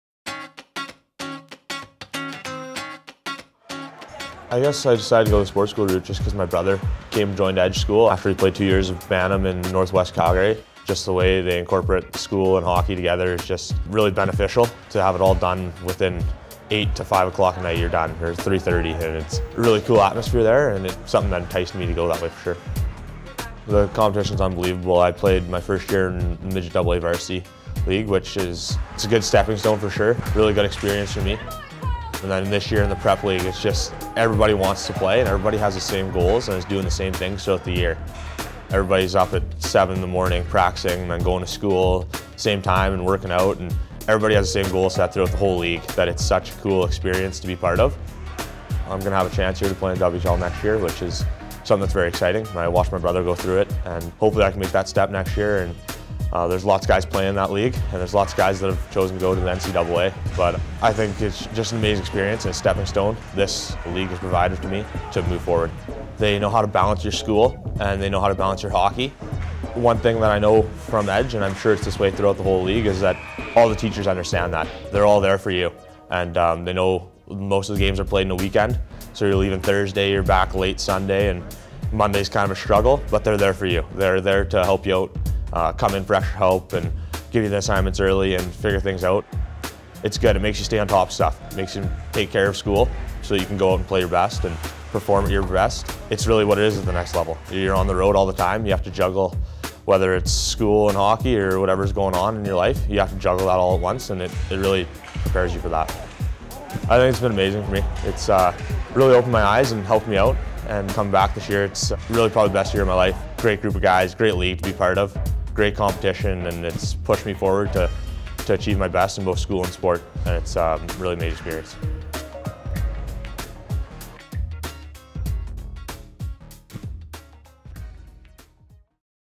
Kyle Dumba | Edge School
2014-15 CSSHL Prep Division Goaltender of the Year and Calgary Hitmen prospect Kyle Dumba of the Edge School for Athletes talks about playing at Edge, in the CSSHL and his long-term hockey goals.